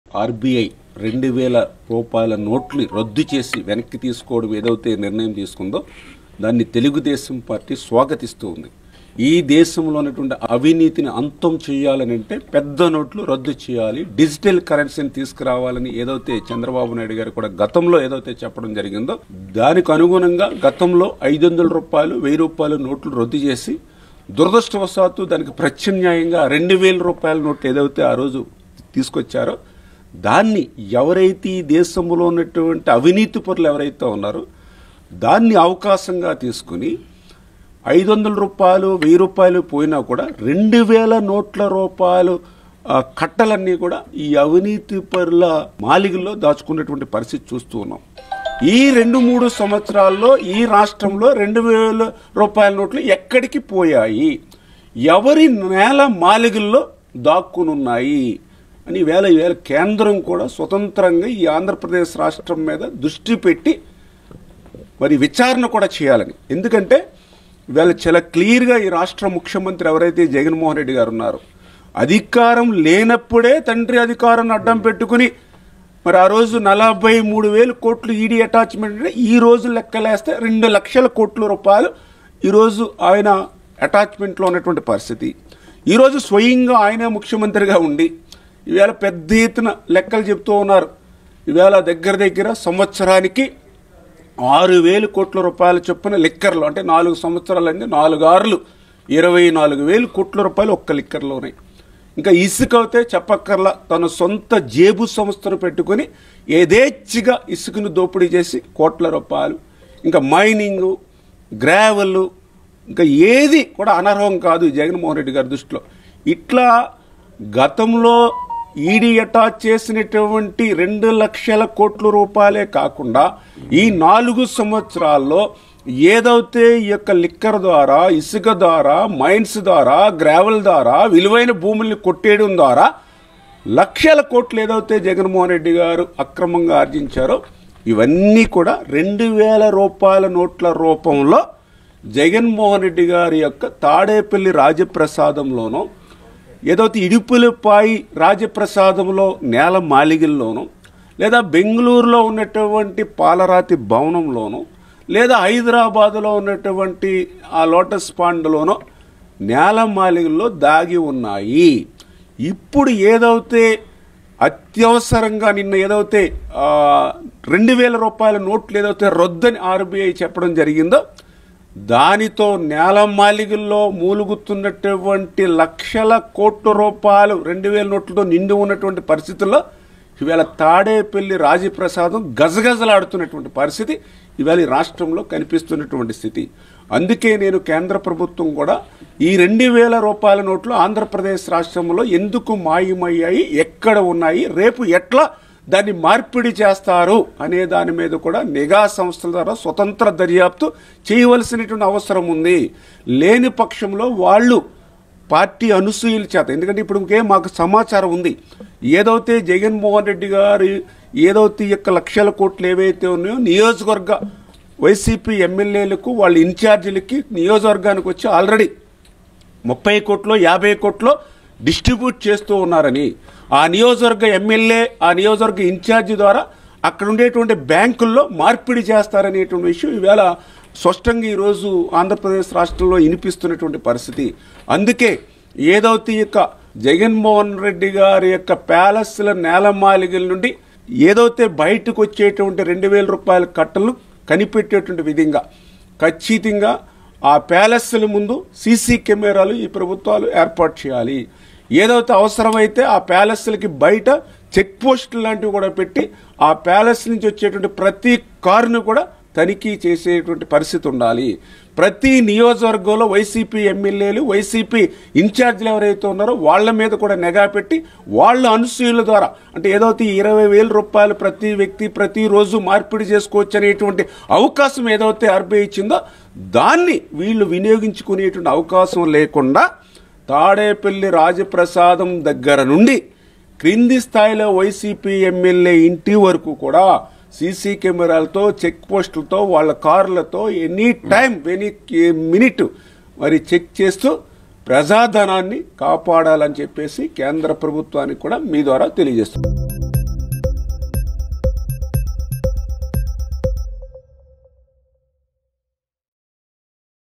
ALL 2000 Notes in Tadepalli Palace | TDP Leader Nimmala Ramanaidu Comments on CM Jagan | Mango News
ALL 2000 Notes in Tadepalli Palace. TDP Leader Nimmala Ramanaidu Comments on CM Jagan.